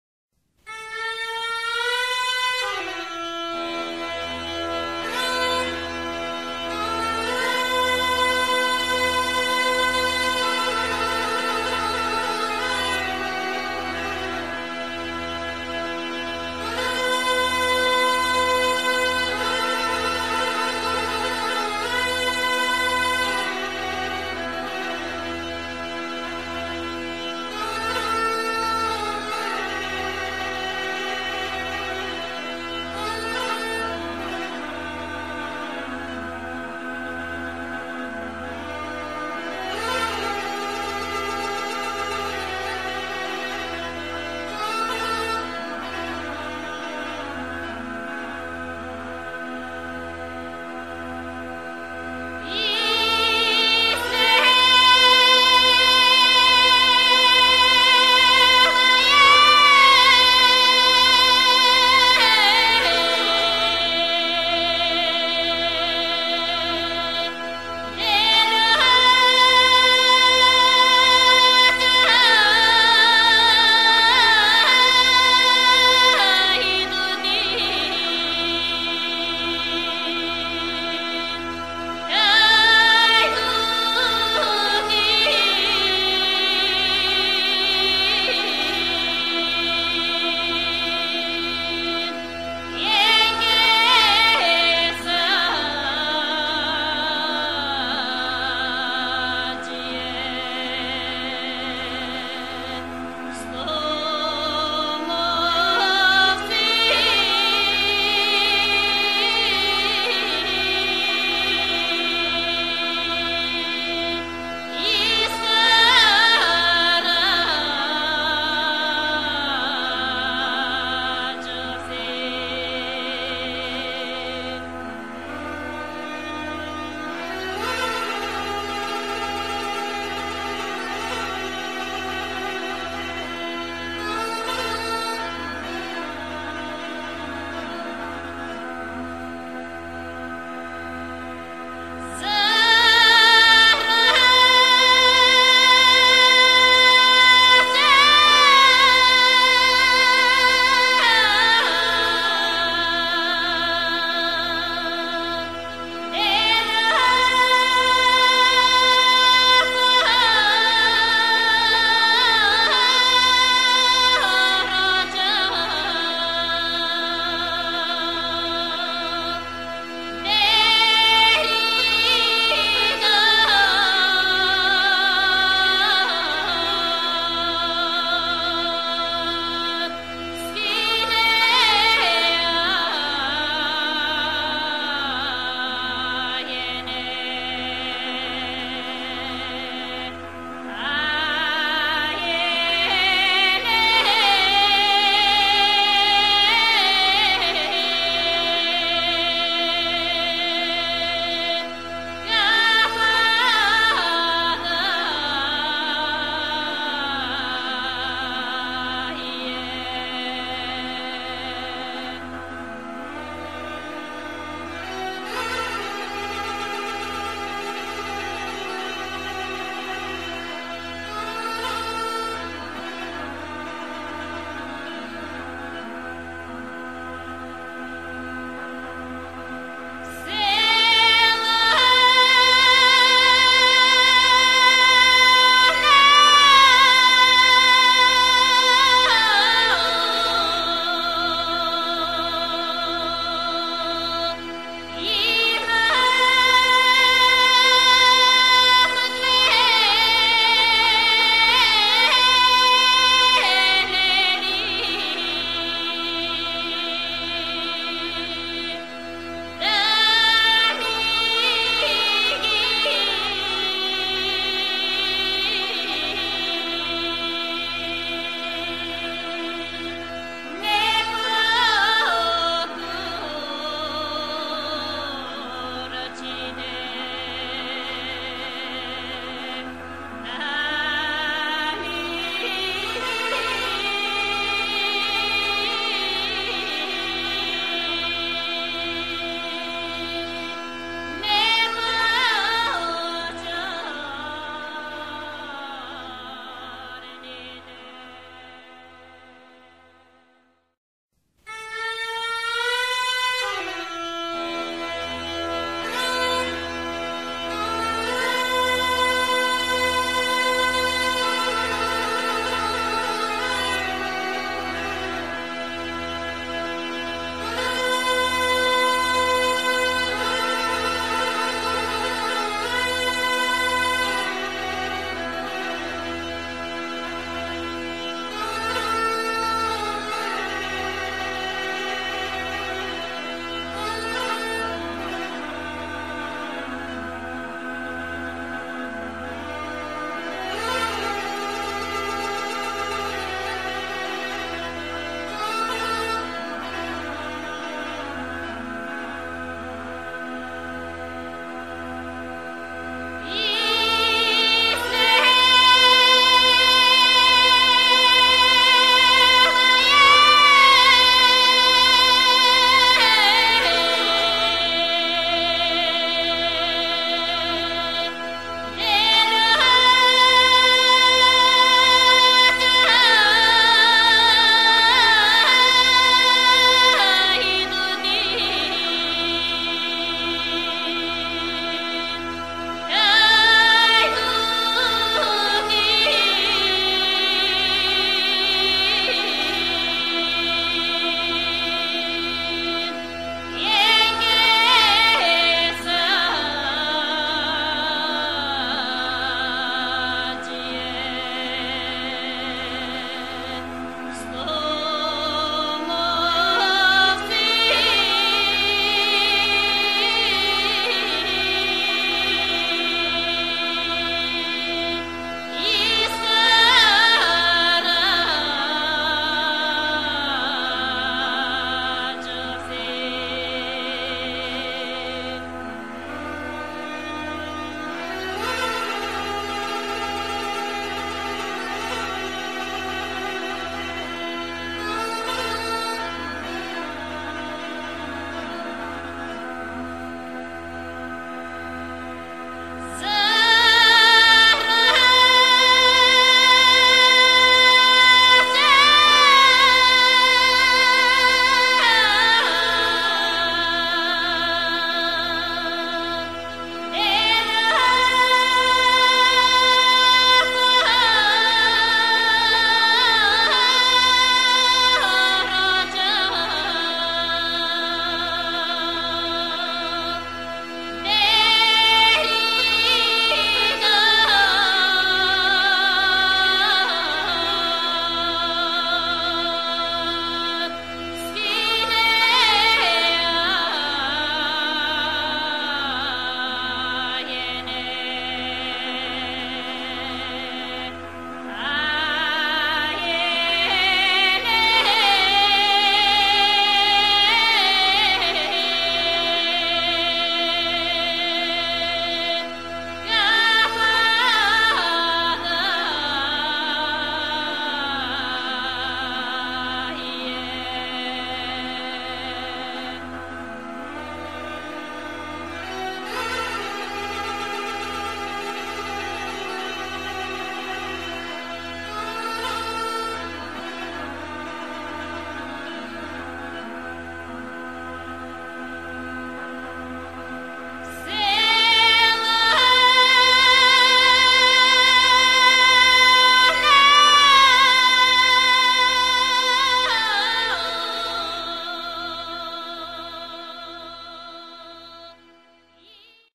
Тибетская Астрология
Коротко о Тибетской Астрологии - почти незнакомая часть необъятного знания Тибета.